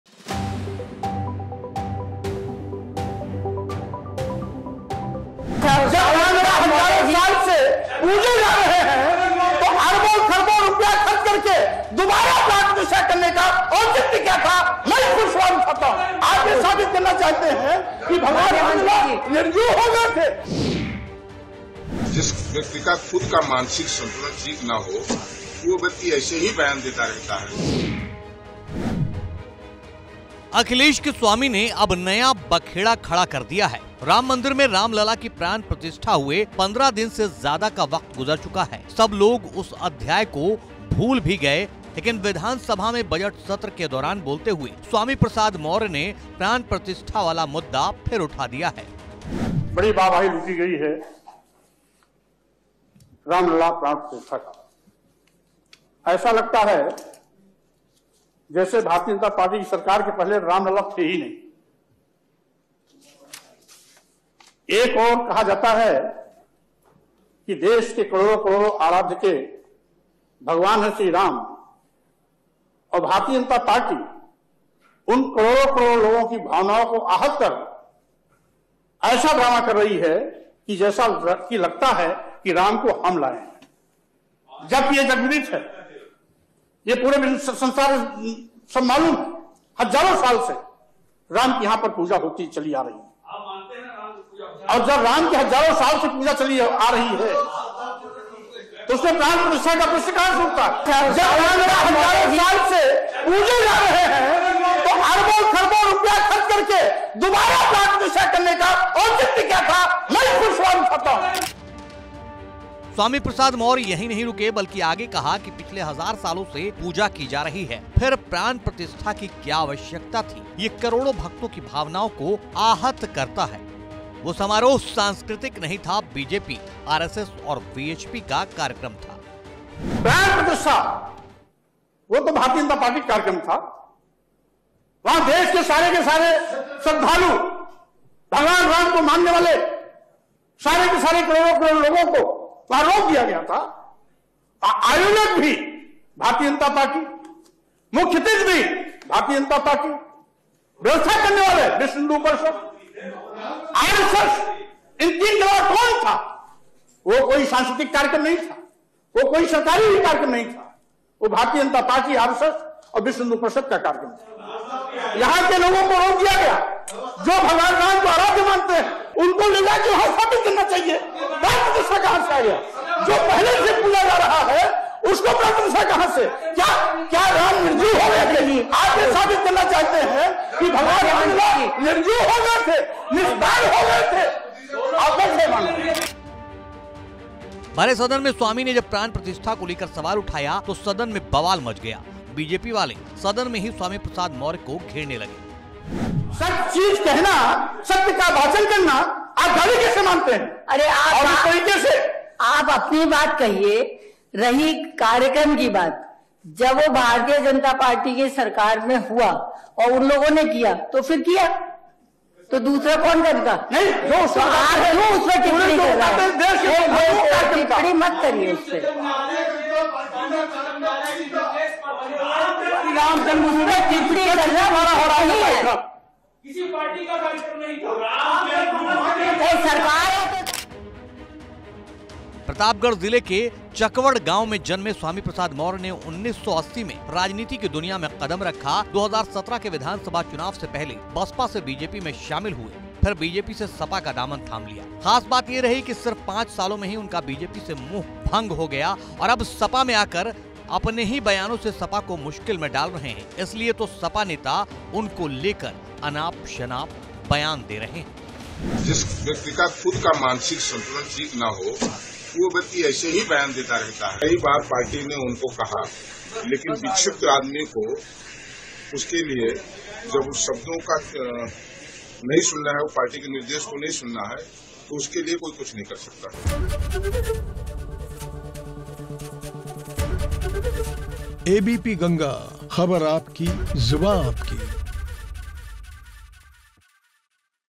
UP Politics : Swami Prasad Maurya ने अब तो सदन में ही प्राण प्रतिष्ठा पर सवाल उठा दिया | ABP GANGA
अखिलेश स्वामी ने अब नया बखेड़ा खड़ा कर दिया है... राममंदिर में रामलला की प्राण प्रतिष्ठा हुए 15 दिन से ज्यादा का वक्त गुजर चुका है... सब लोग उस अध्याय भूल भी गए... लेकिन विधानसभा में बजट सत्र के दौरान बोलते हुए स्वामी प्रसाद मौर्य प्राण प्रतिष्ठा वाला मुद्दा फिर उठा दिया है